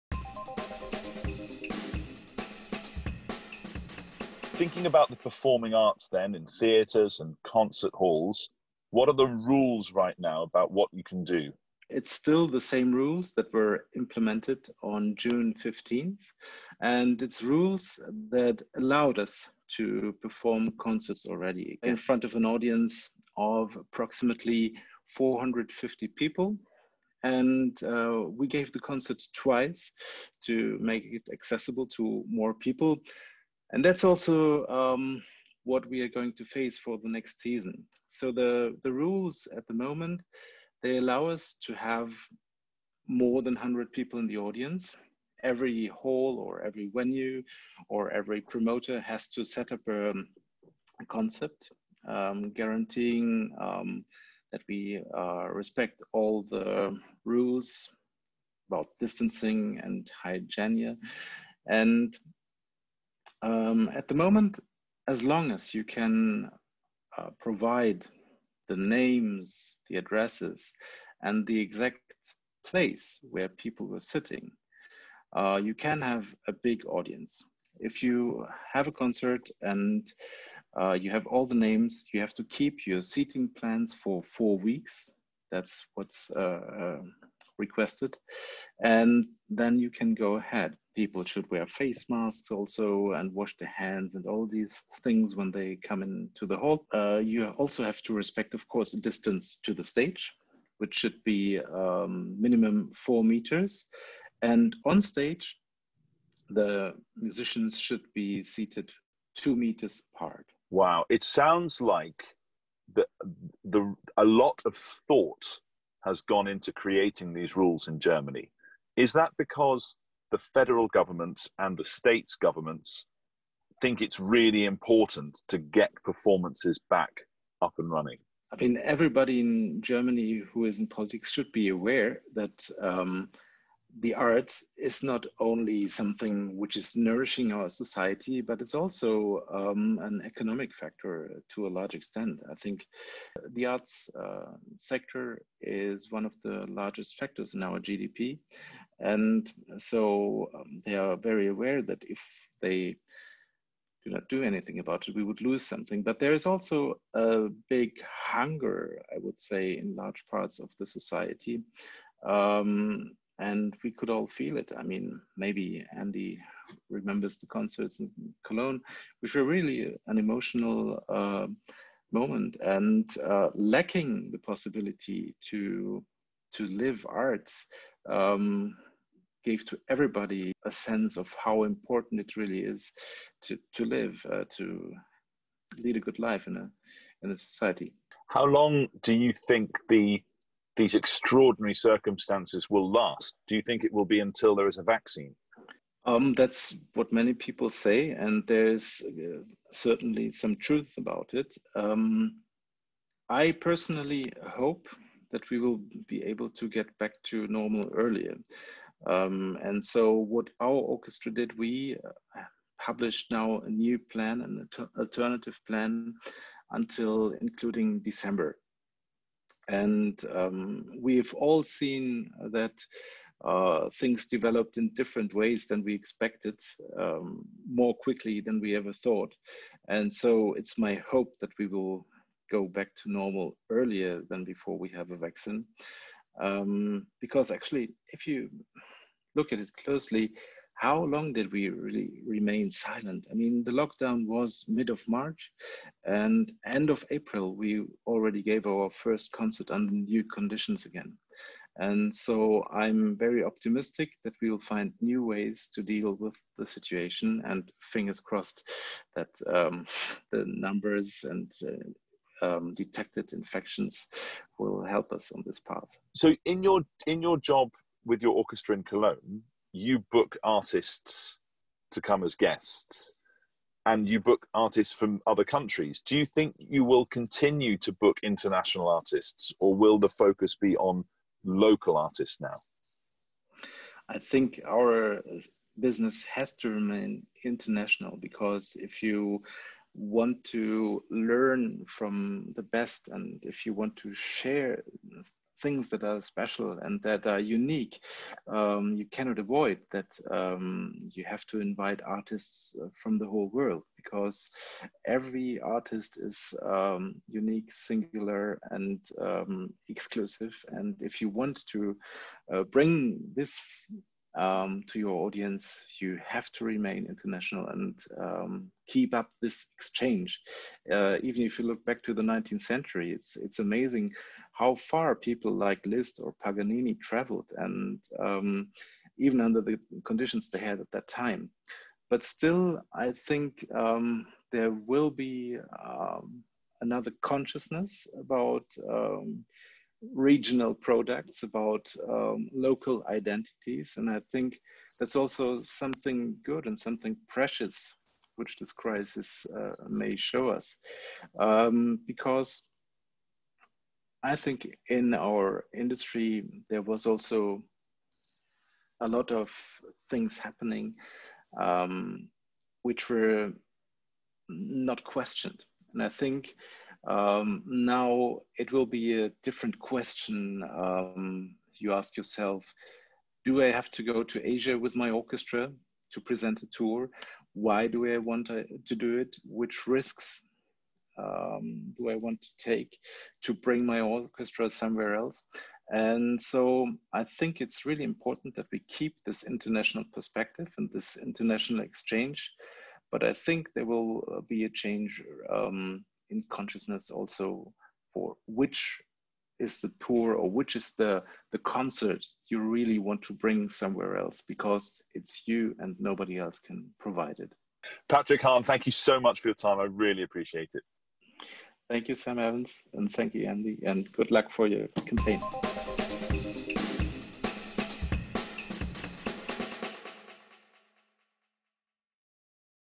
International Perspectives - Patrick Hahn, Gürzenich Orchestra talks to OneVoiceCampaign
Patrick Hahn - Head of Artistic Planning at the Gürzenich Orchestra talks to Sam Evans and explains the situation in North-Rhein-Westfalia right now.